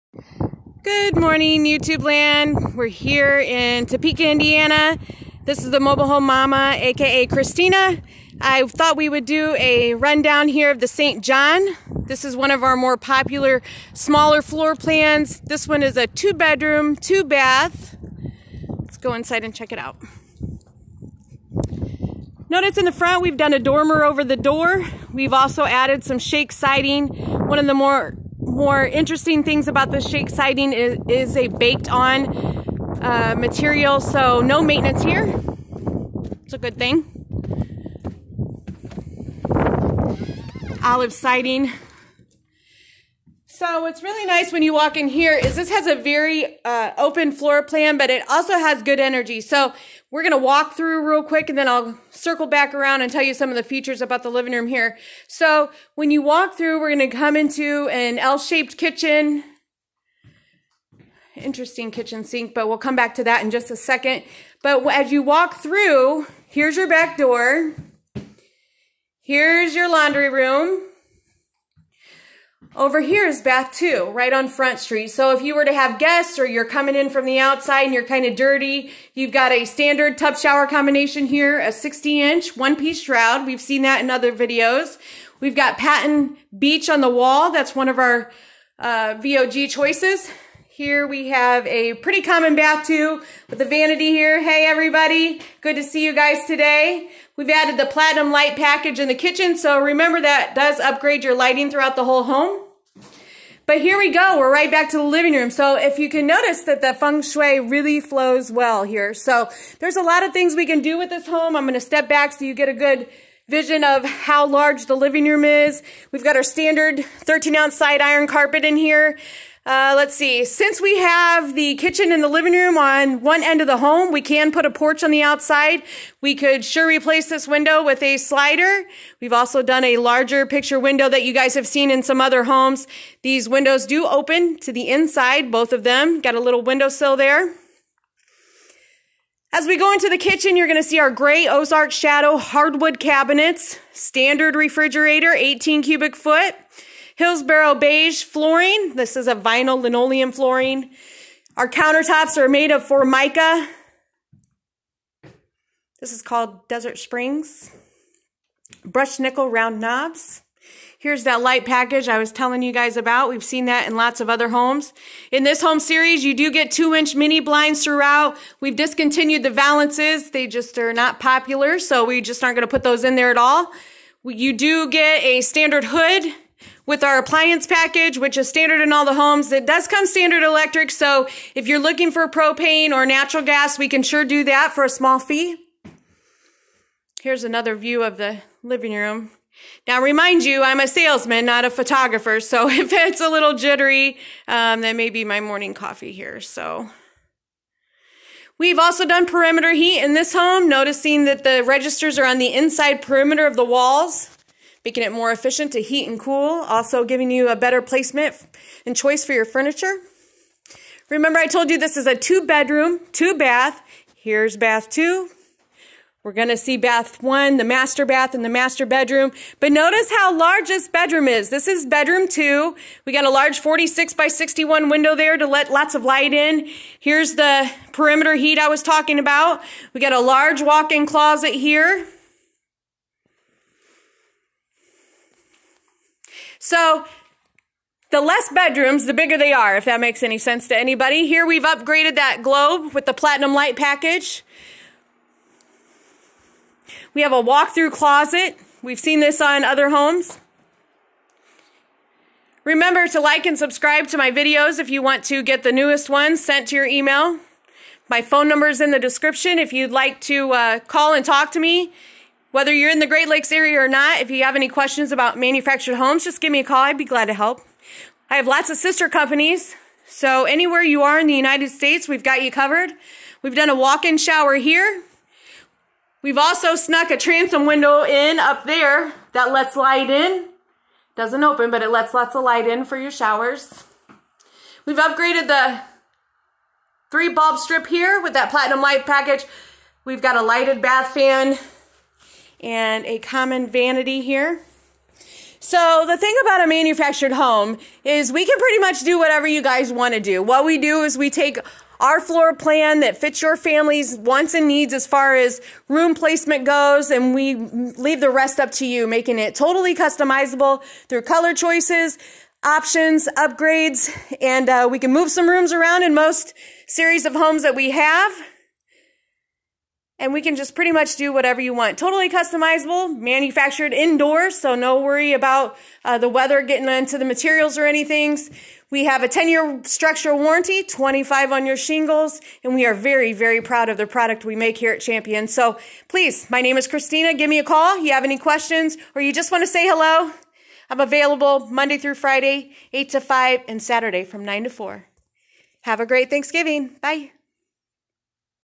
St. John 2846 201 By Champion Homes
This is a walk through of the St. John home by Champion. It is shown as a 2 bedroom, 2 bath manufactured home with great feng shui. It is a 28X46 available in a 2 or 3 bedroom version. This mobile home is sure to fit any budget. Please call for more information. I will provide a FREE night's stay for you and your family to view our model village.